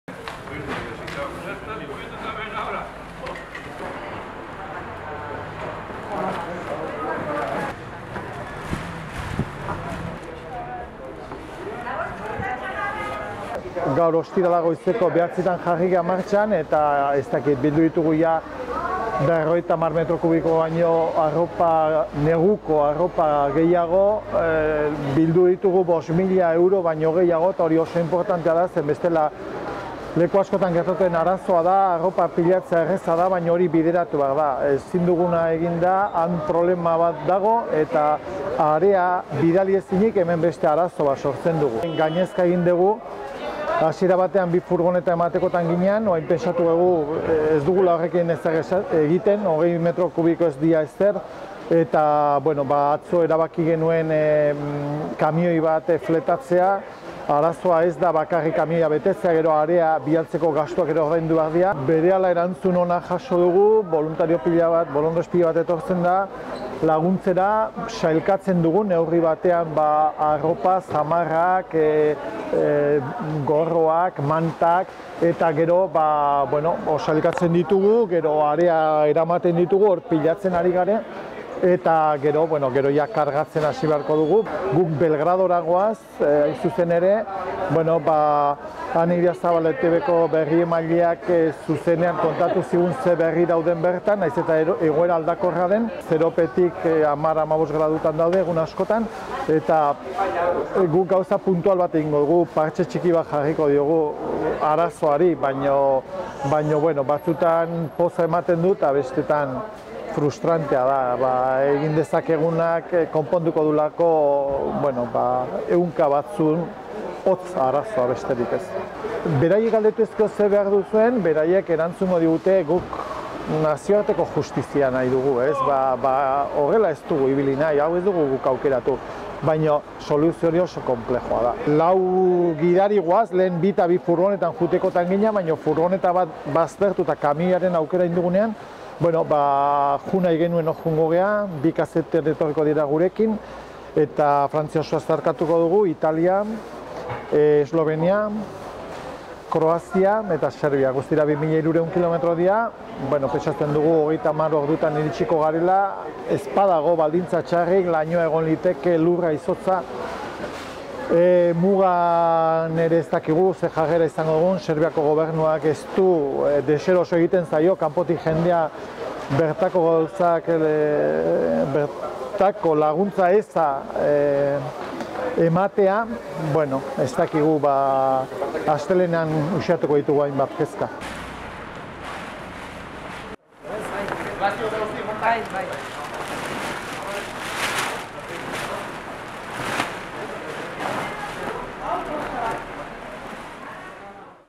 Neguko arropa bilketa Belgradeko errefuxiatuentzat
| Zero azpitik hamar graduko tenperatura dute egunotan Serbian. Egoera horretan bizi dira 7.500 errefuxiatu. Igandean abiatuko da Tolosatik Belgraderuntz lagun talde bat, neguko arropa eraman nahi diete.